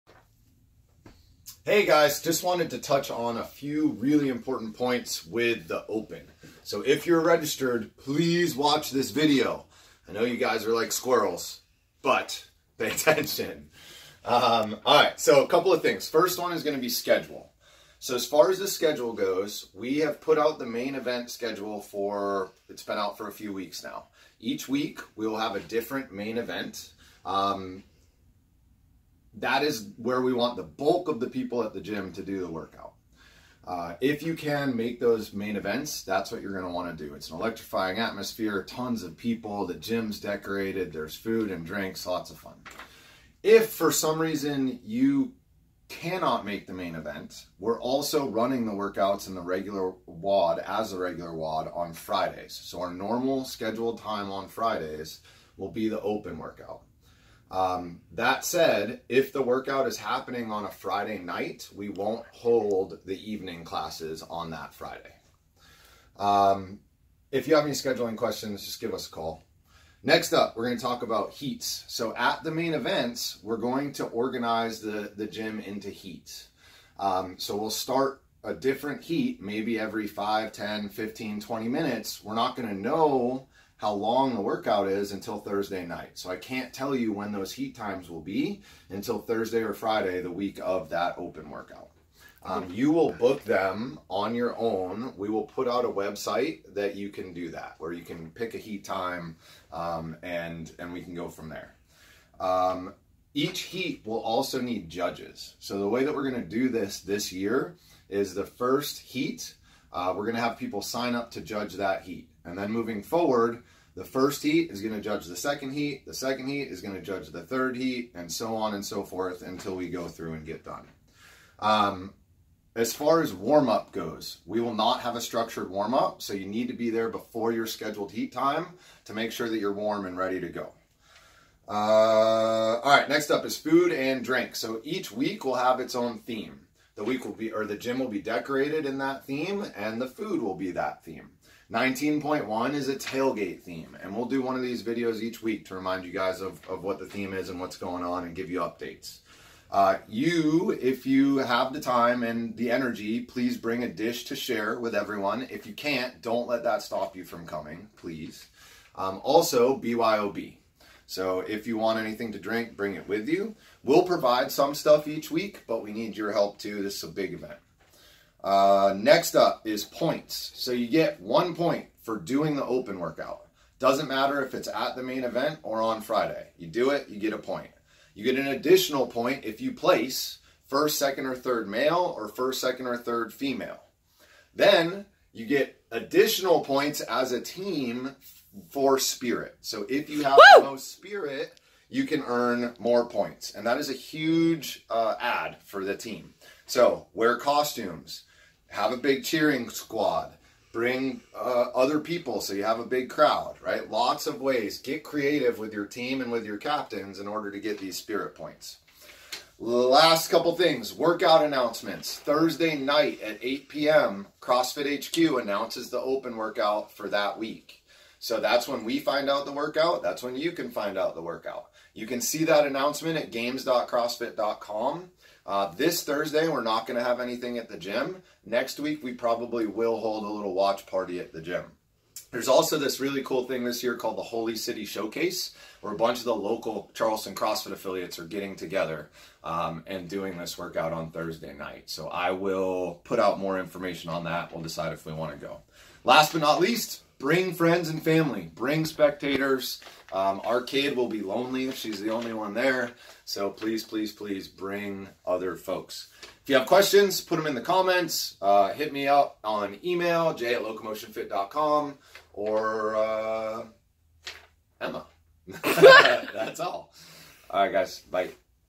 The Locomotion Intramural Open 2019
Details of how The Open at Locomotion Fitness works this year! If you have any questions or if this is your first Open - WATCH THIS!